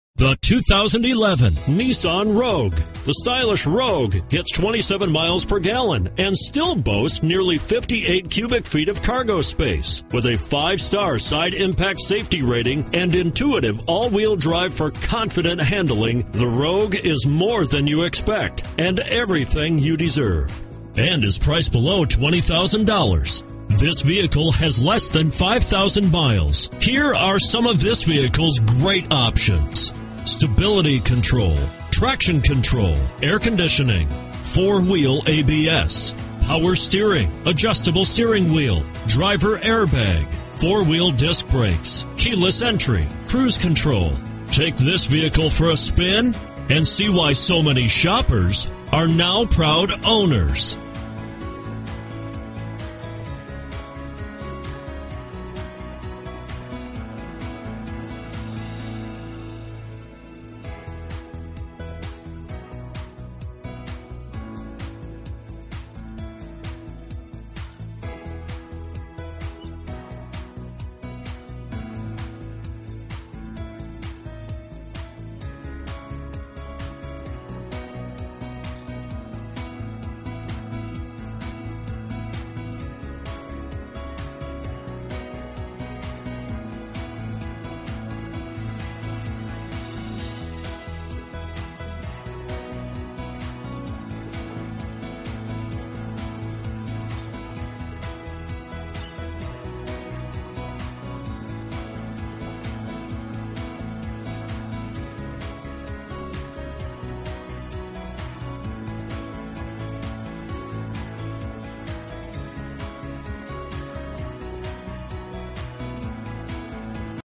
2011 Nissan Rogue - Sport Utility San Antonio TX, Used N1120
2011 Nissan Rogue  - Stock#: N112034A

Gunn Nissan
12838 San Predro

Very Low Mileage: LESS THAN 5k miles. It just doesn't get any better!! This Rogue is simply quality in every aspect. One of the finest cars around, you won't believe what you get for the money! Nissan CERTIFIED!!! This gas-saving SUV will get you where you need to go... Does it all!! New Inventory... Safety equipment includes: ABS, Traction control, Curtain airbags, Passenger Airbag, Stability control...Relax in the comfort of features like: Power locks, Power windows, CVT Transmission, Air conditioning, Cruise control...Nissan CERTIFIED!!!